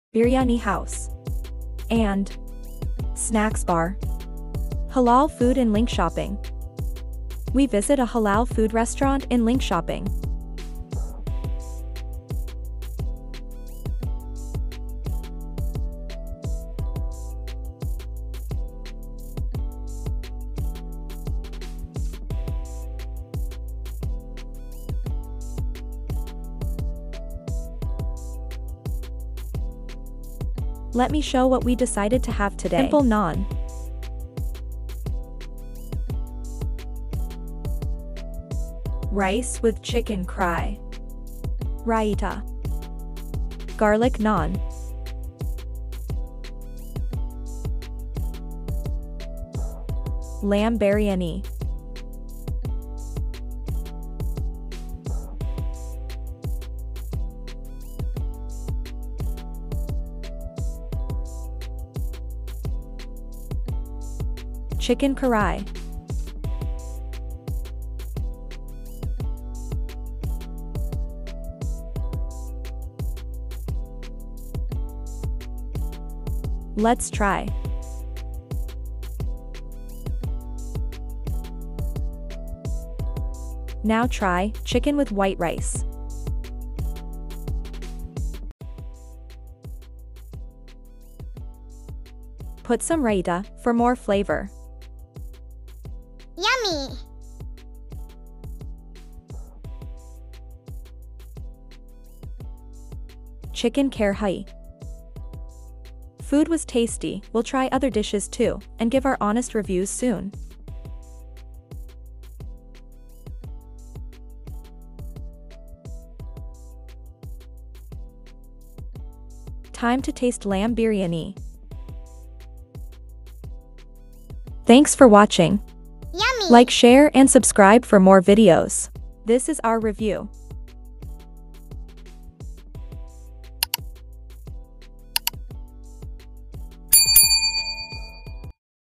Halal food in Sweden| linköping
Visit to a Halal food in Linköping Sweden 🇸🇪. Food was good in taste and environment and vibes are also good we will try more and  will review more.